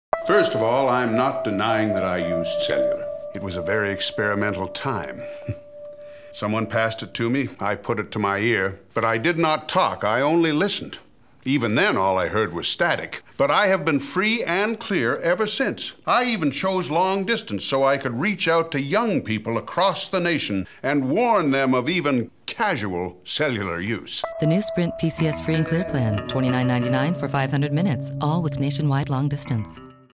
Bad Congressman - Sprint PCS
Sprint PCS - Congressman

Shady politician chats up the meaning of cell plans.